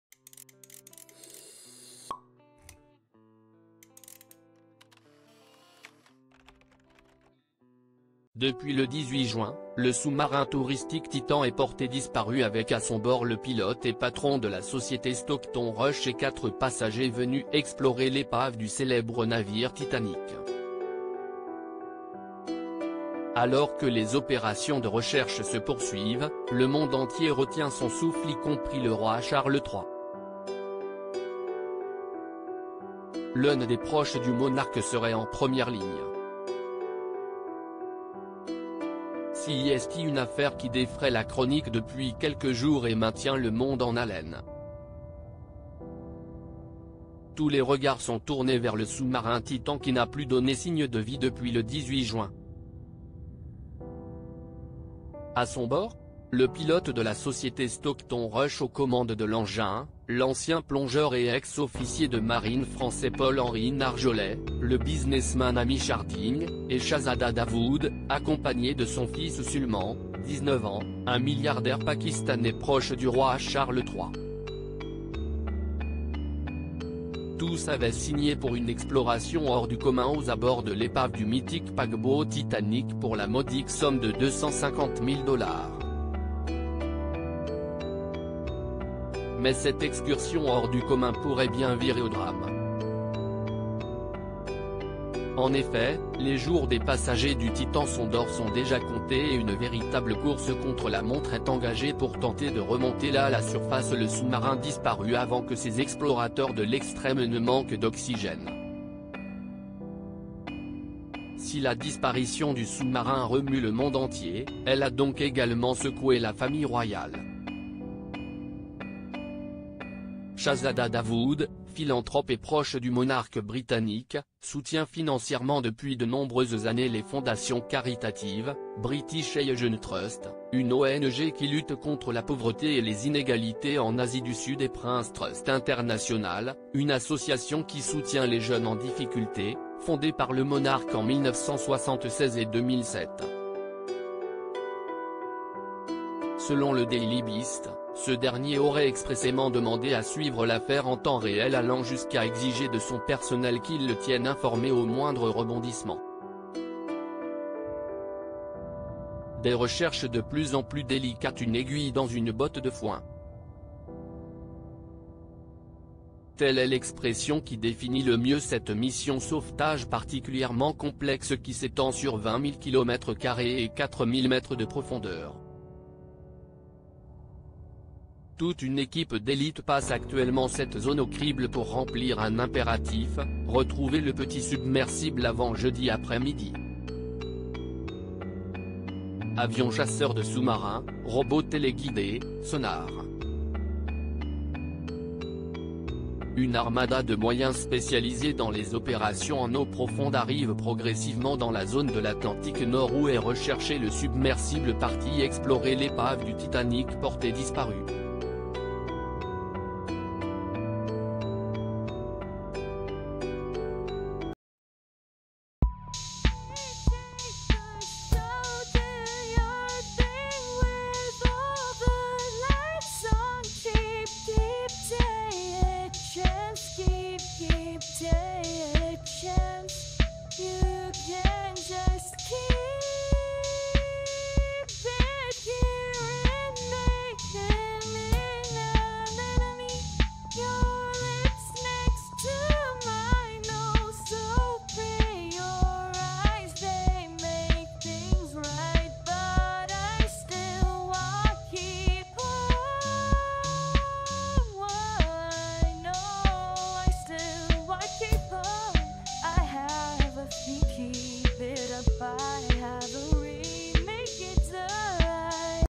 Sous-marin disparu près du Titanic : Charles III aux abois, l'un de ses proches à bord du submersibl
Sous-marin disparu près du Titanic : Charles III aux abois, l'un de ses proches à bord du submersible...